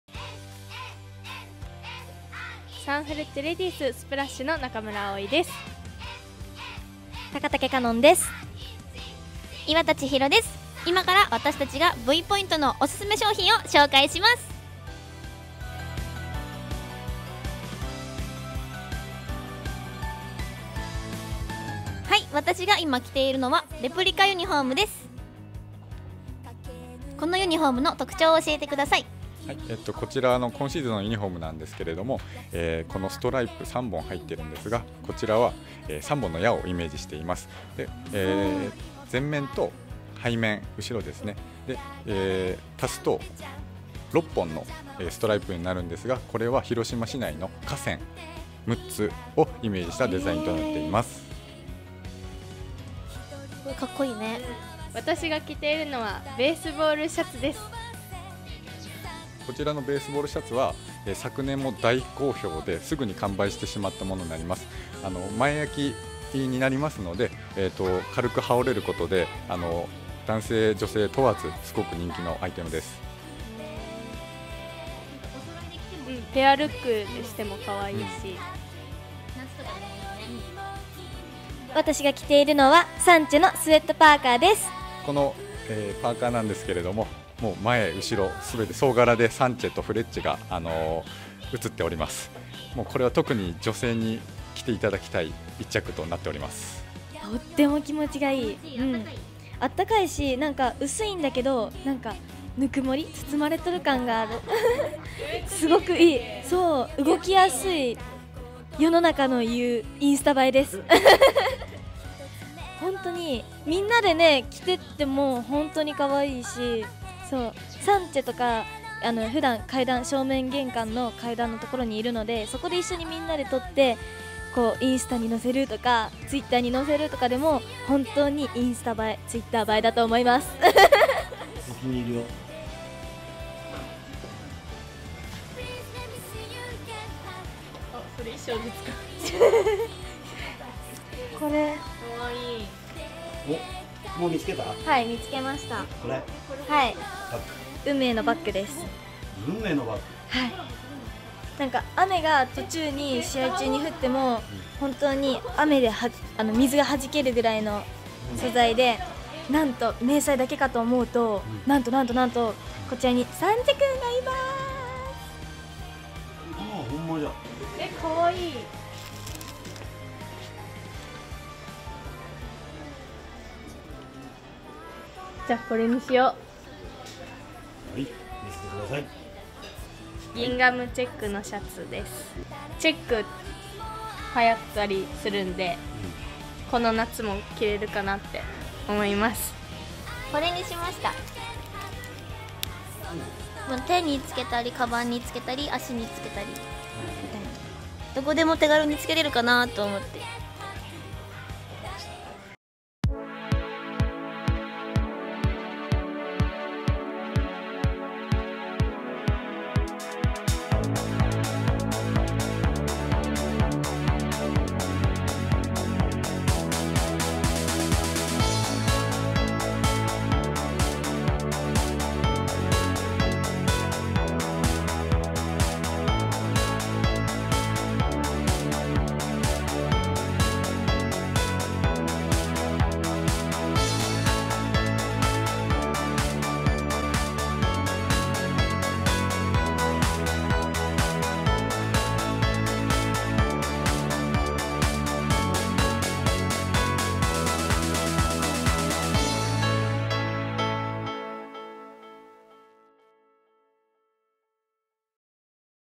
サンフレッチェレディースがおすすめ！2018シーズンサンフレグッズ
サンフレッチェ広島　オフィシャルショップV-POINTは、
紙屋町シャレオで年中無休で営業しています！

この春のおすすめグッズを、サンフレッチェレディース「SPL∞ASH」の皆さんに紹介していただきました♪